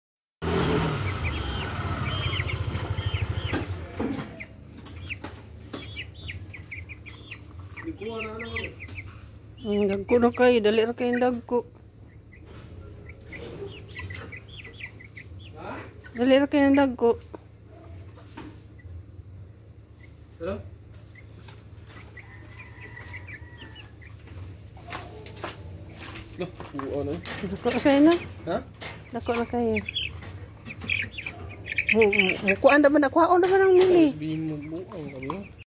Our chicken growing fast
Feeding and watering the chicken  food time and drinks